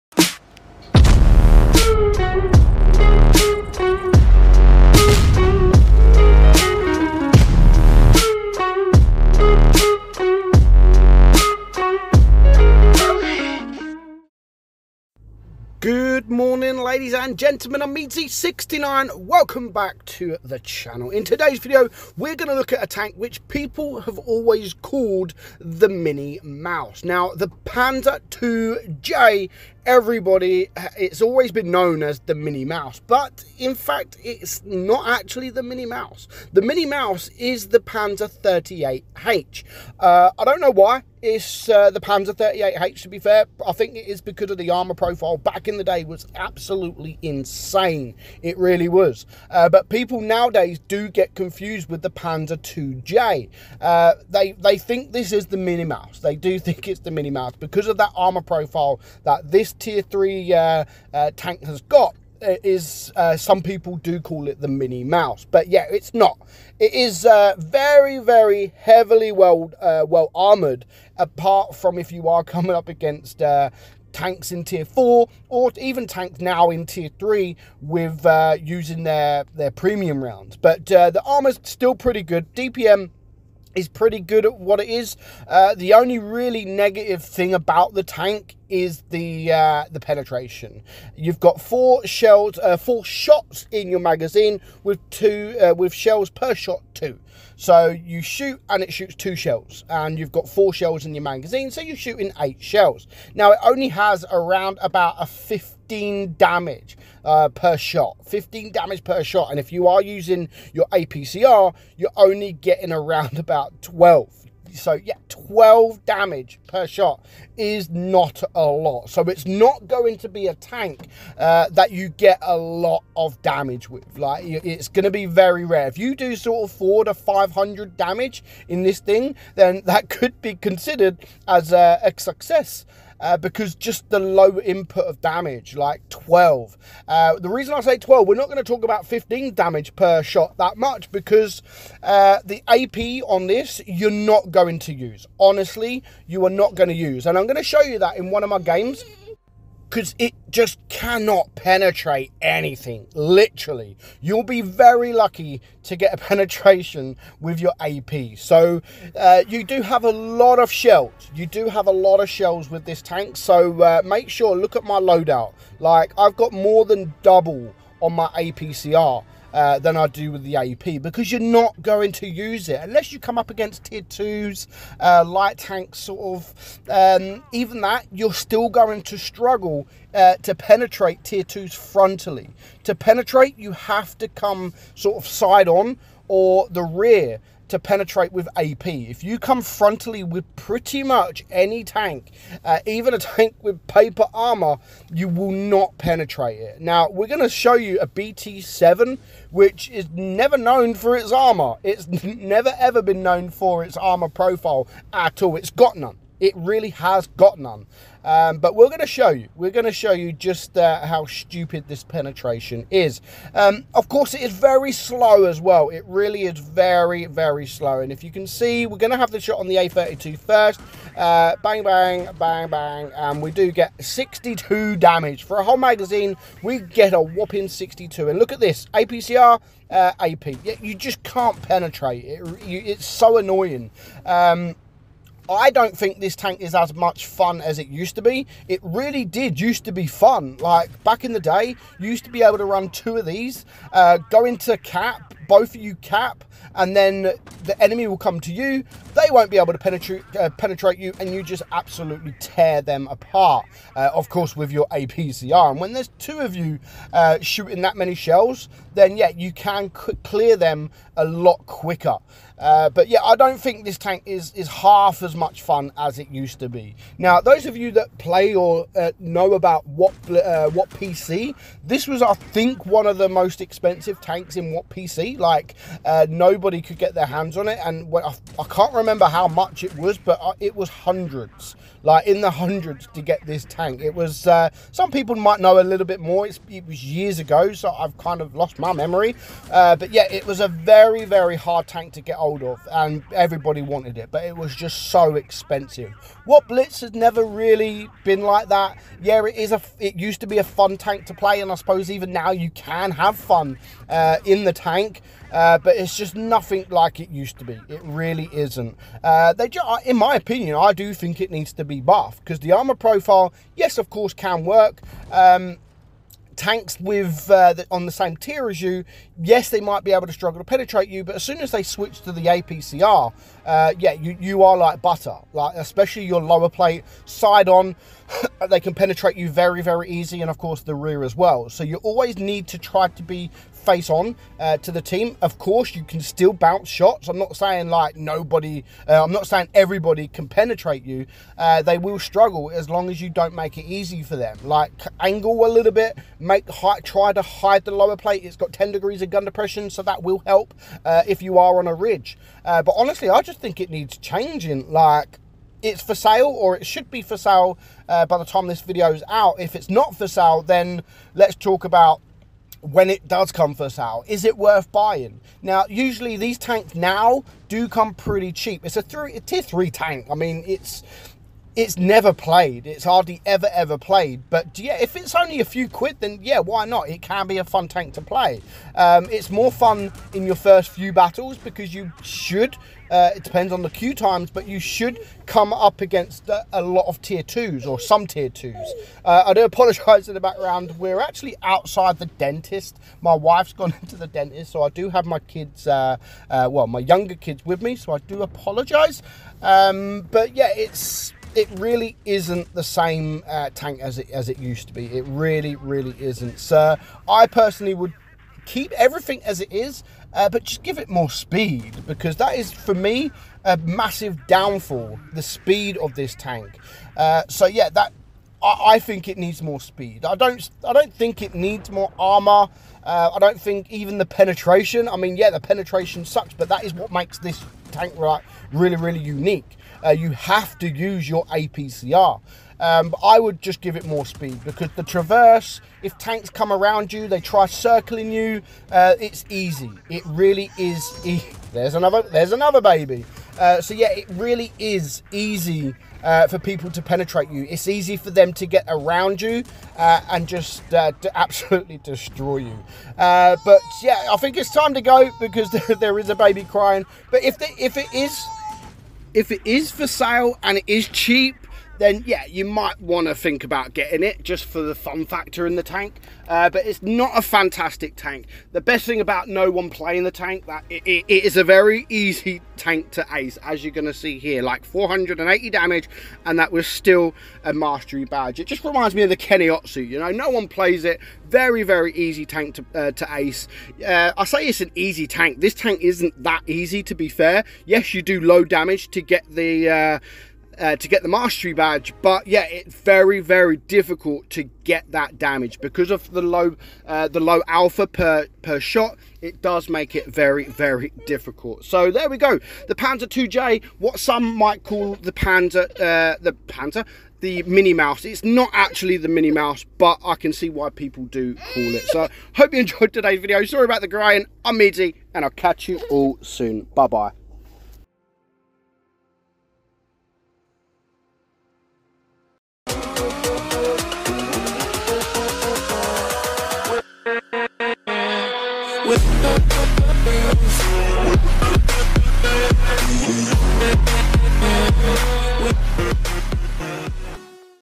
The Mighty Miniature MAUS
In todays video we play what some people call the Mini Maus. it's not actually the real mini maus but you can see why some call it that...because of that armour! is the tank worth having in your garage?